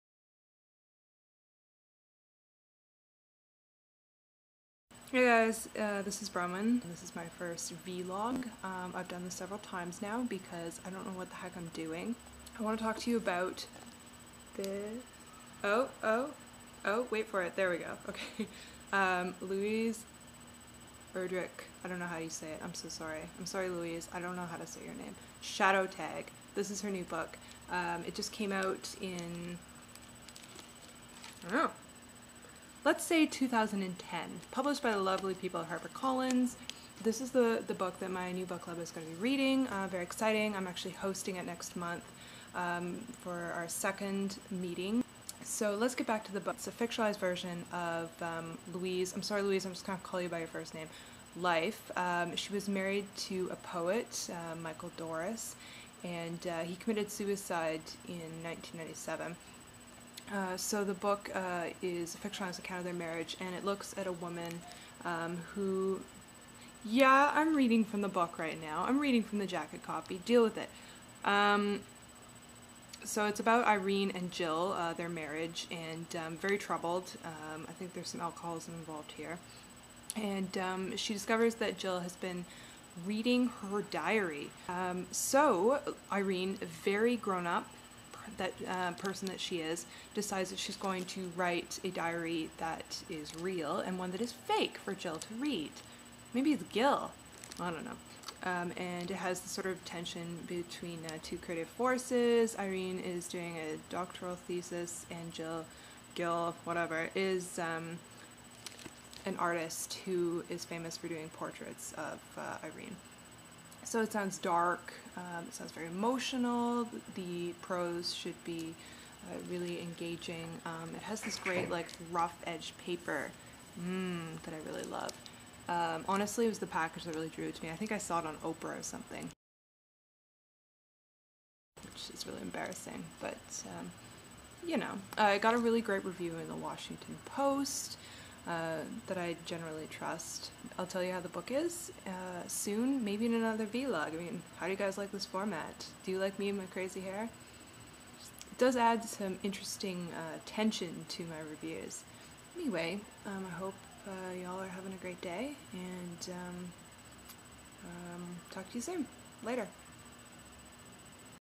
Shadow Tag_Vlog_Webreadyfinal.mov
An introduction to Louise Erdrich's Shadow Tag, my next book club pick.